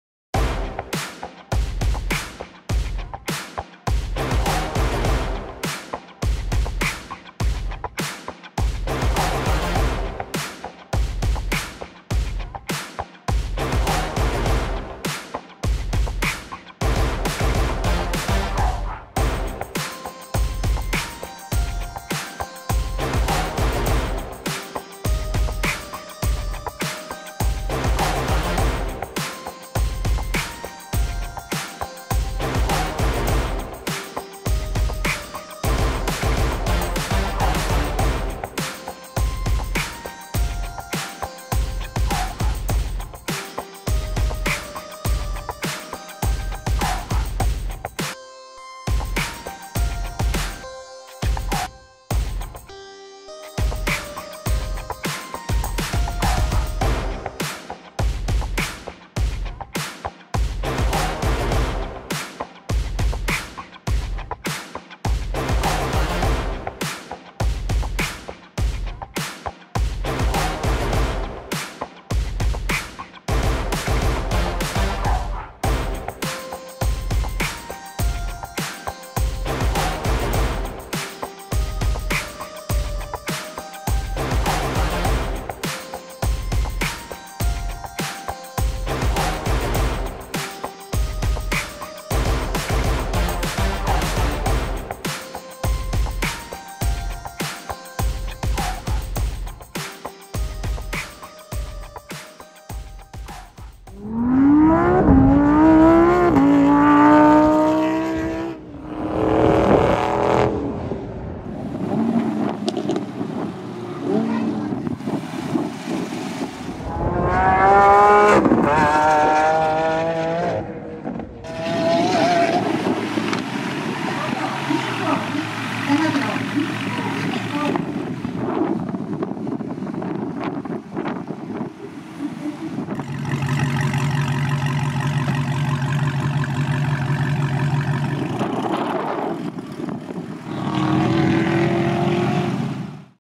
ランボルギーニとアメ車が大阪・舞洲に集まる！ミツオカ サマーフェスティバル 2022｜Lamborghini, Dodge, Corvette, Mitsuoka, Osaka, JAPAN
光岡自動車主催の「ミツオカ・サマーフェスティバル 2022」の様子です。
光岡自動車の取り扱うミツオカはじめ、ランボルギーニ、ダッジ各モデル、コルベットなどのアメ車が集まって200メートル加速とサウンドを披露しています。